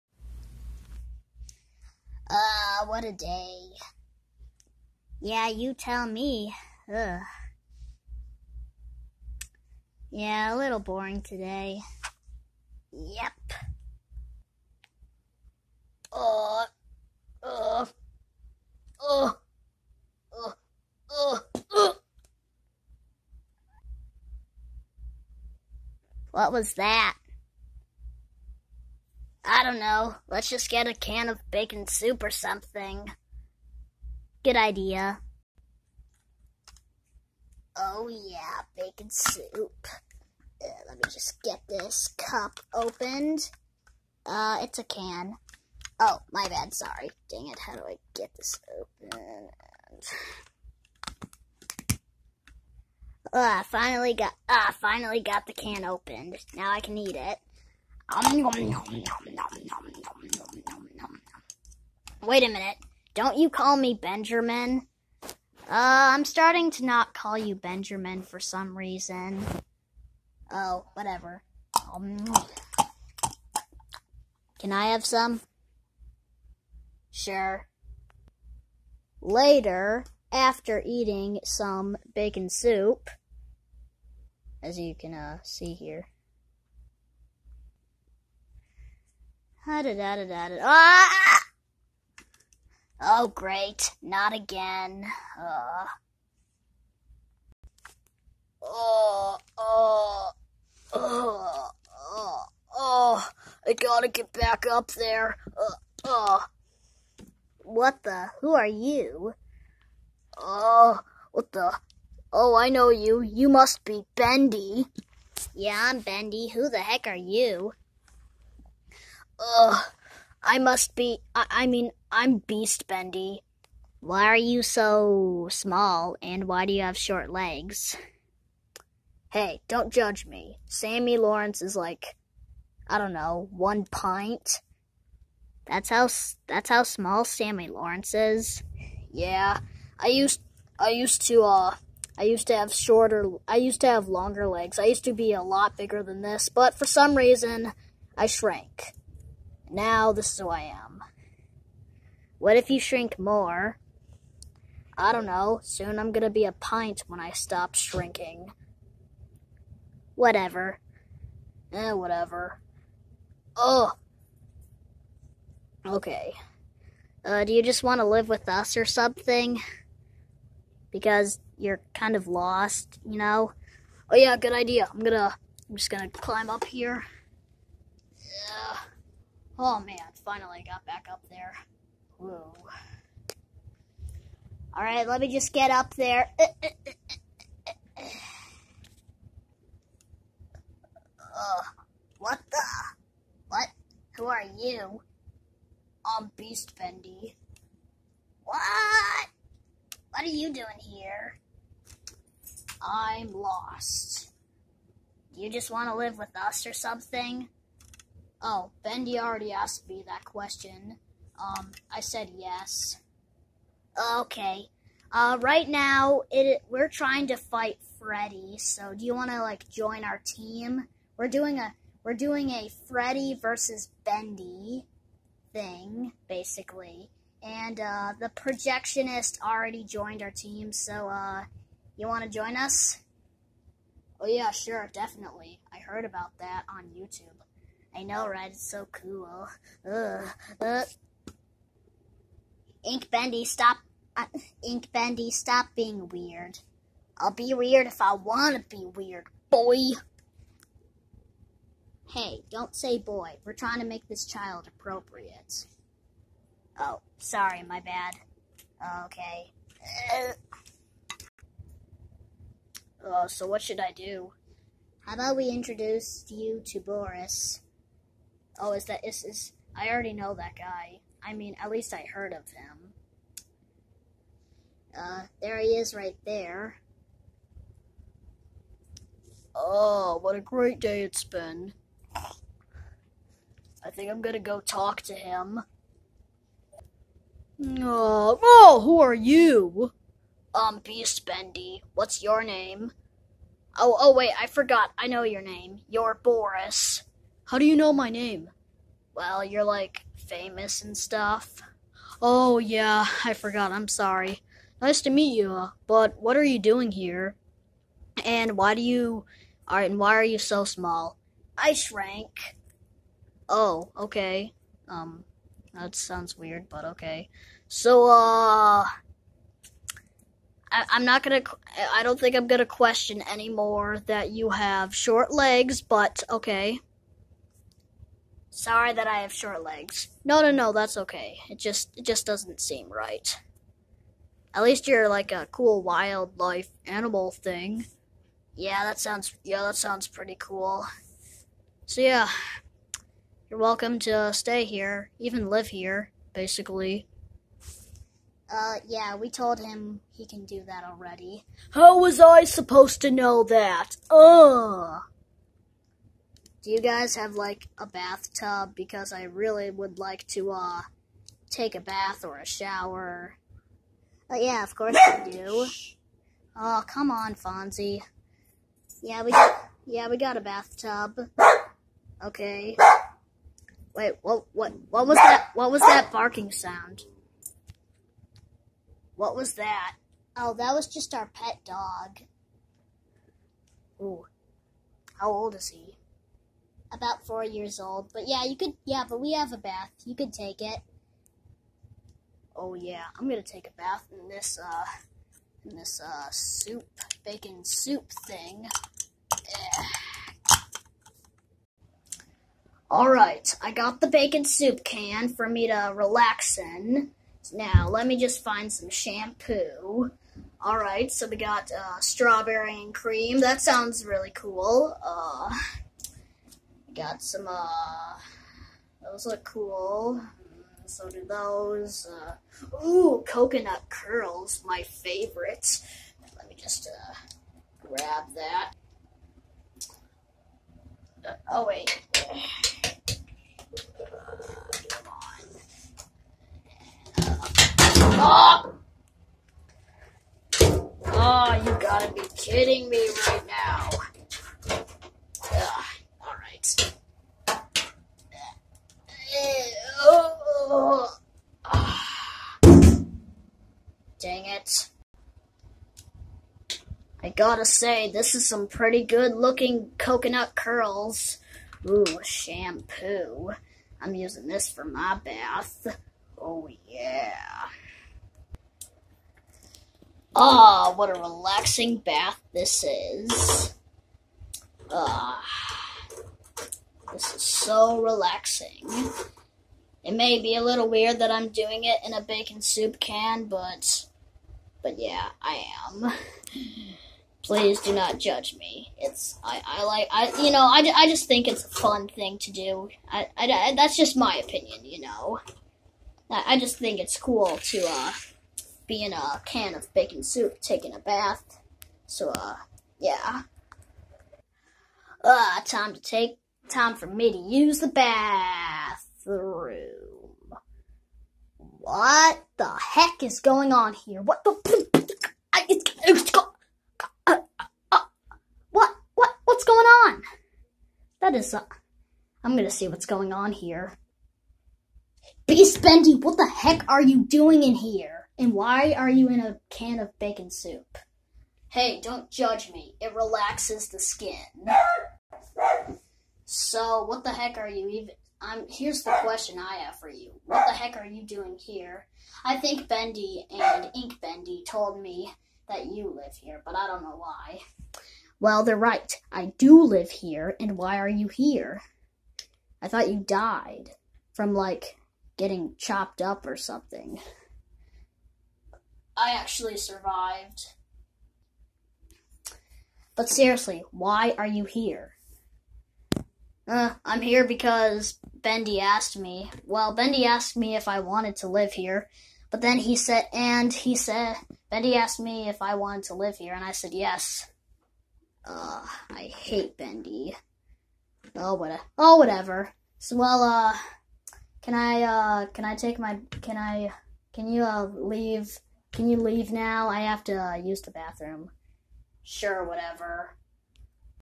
Bendy meets beast bendy. Read description
Sorry if my beast bendy figure is small.